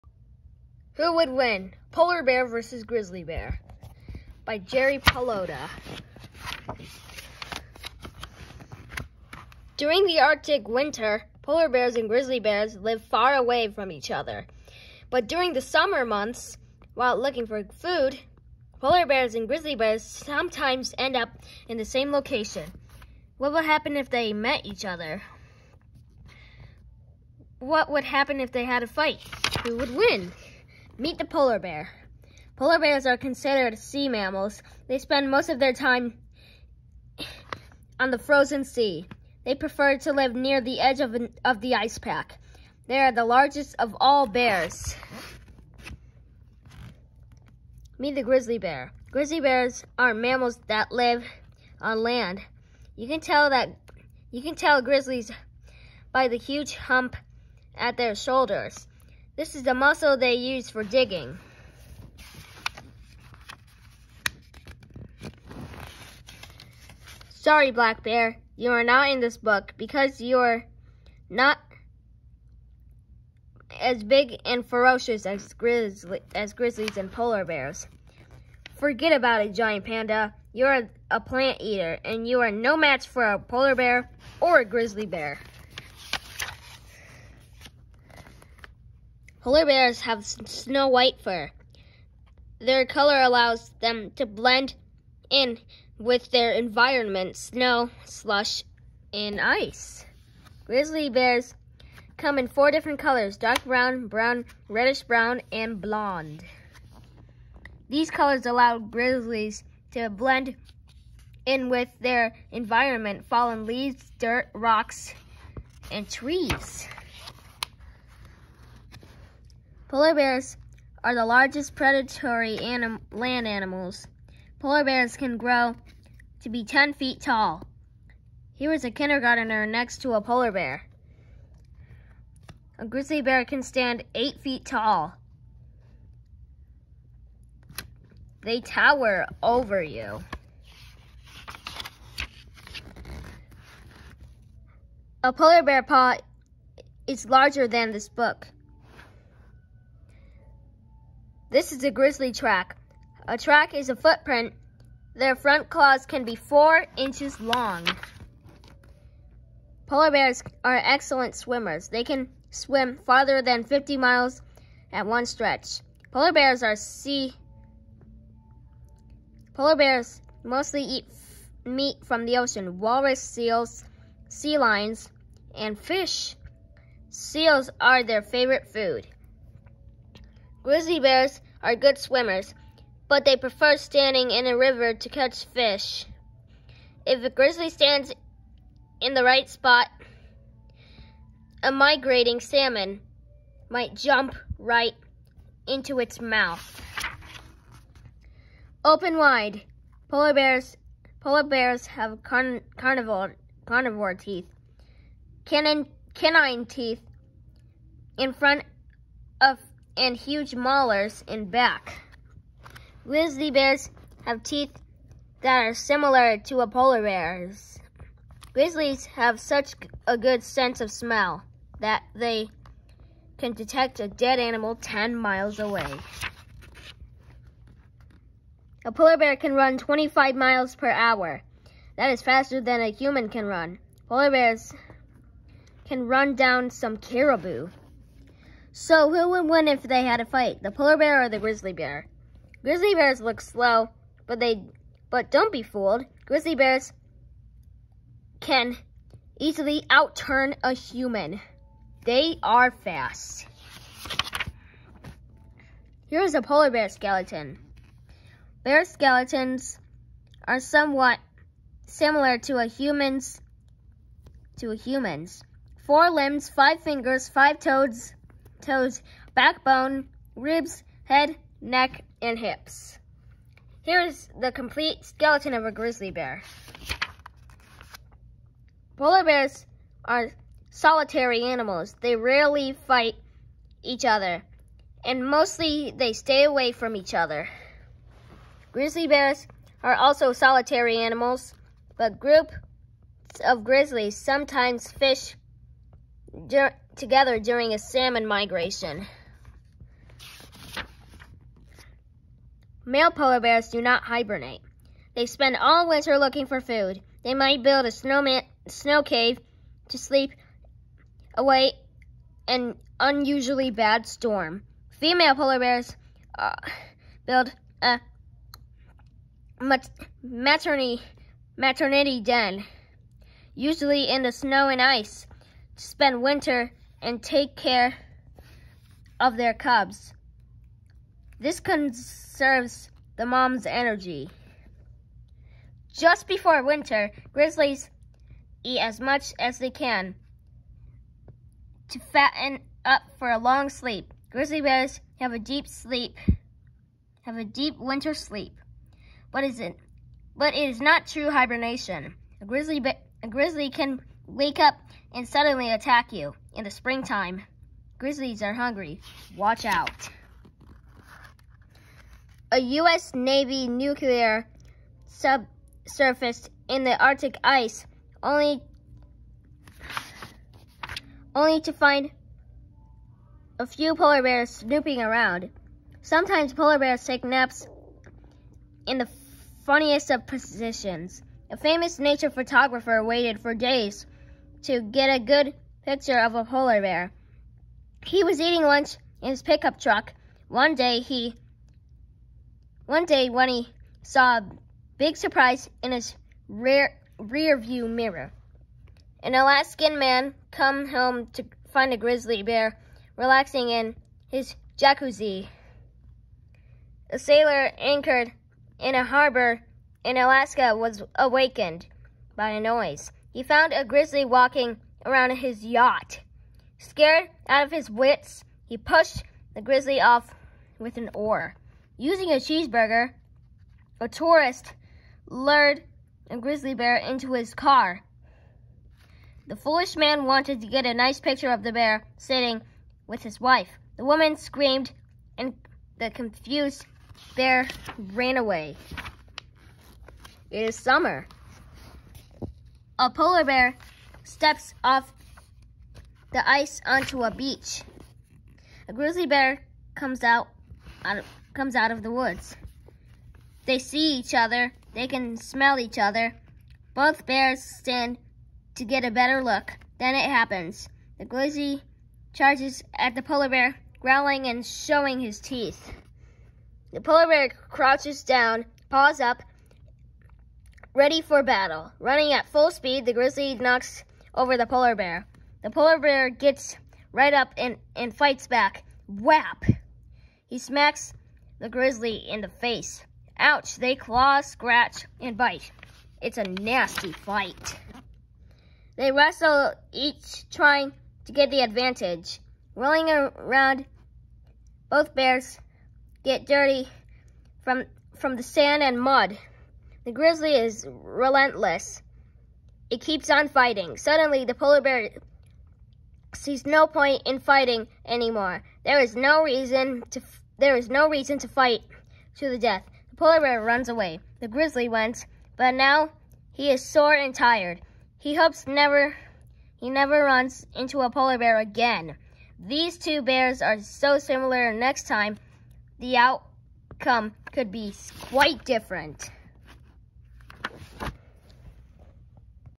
TyGrant09 Reads Who Would Win? Polar Bear VS. Grizzly Bear
The third Who Would Win? Book in the series!